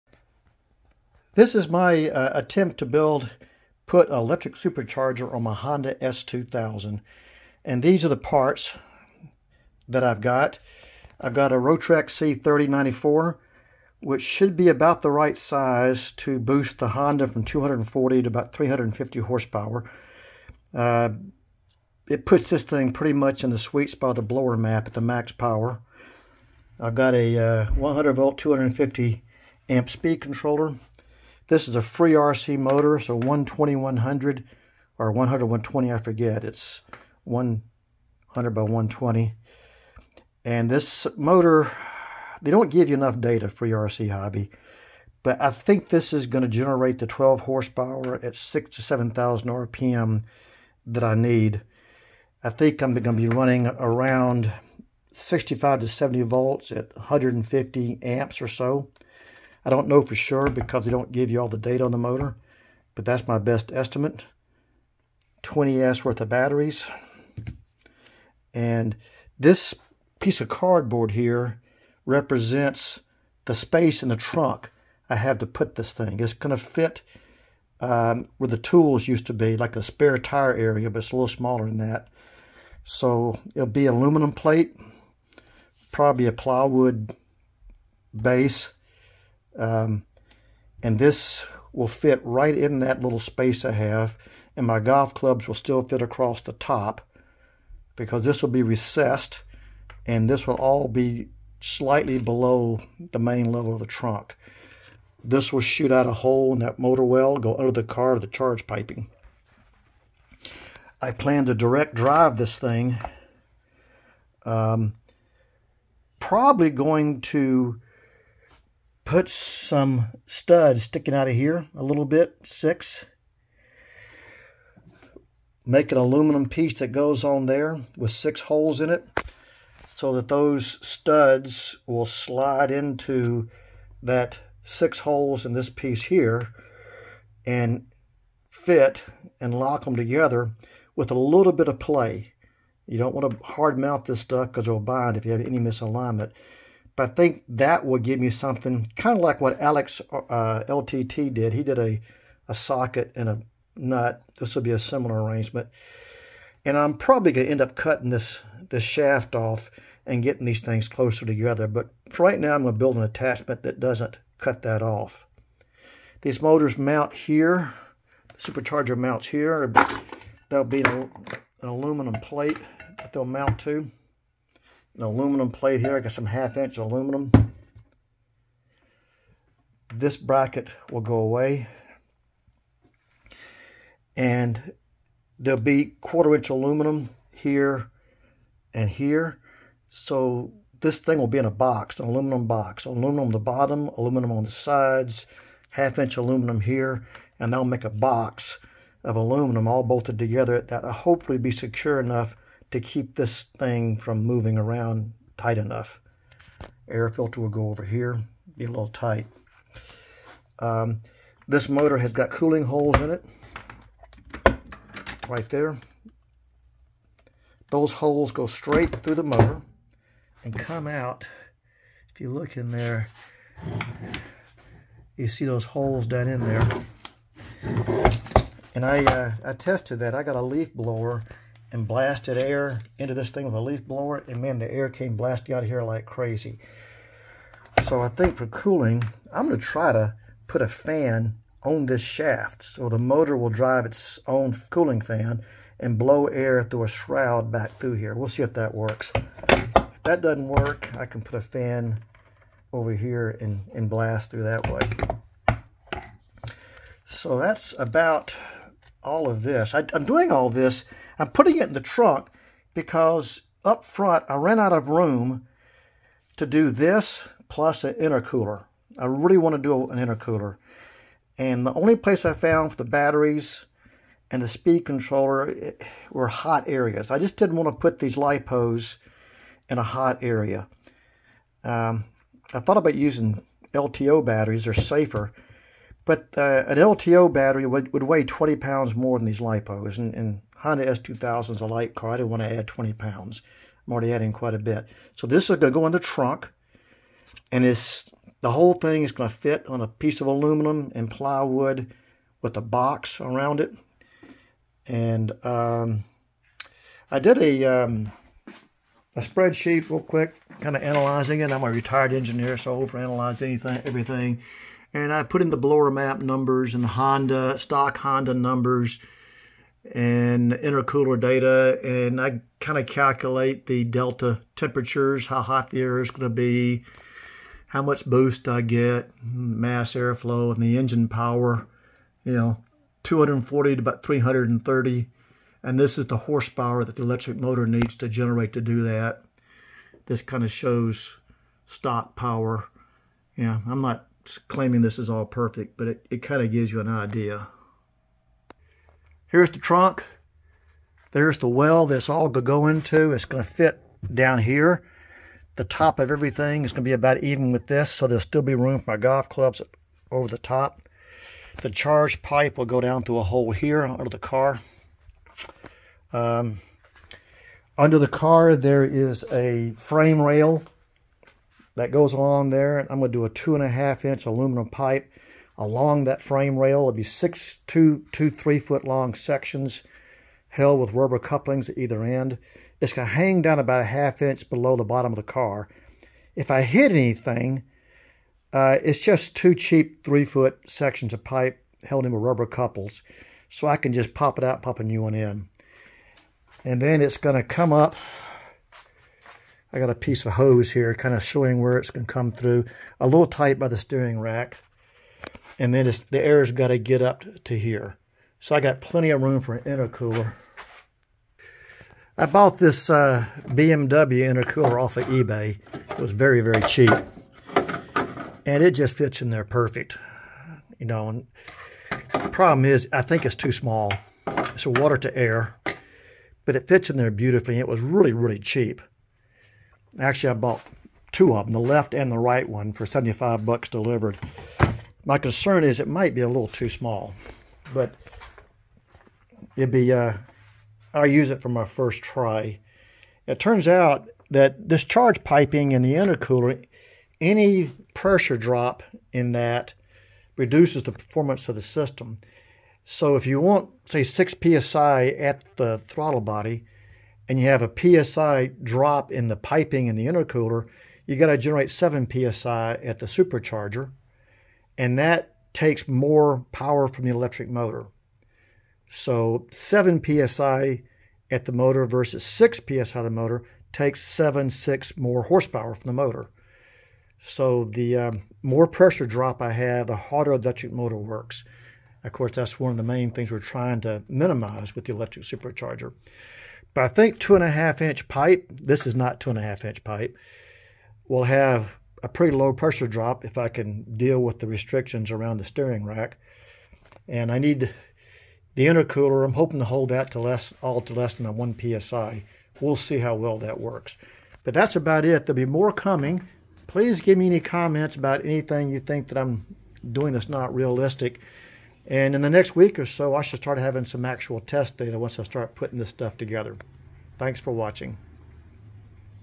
Electric Supercharger for a Honda S2000 - in the trunk
I'm trying to put a Rotrex Supercharger in the trunk of my S2000.  I'm using a FreeRCHobby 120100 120kV  Motor.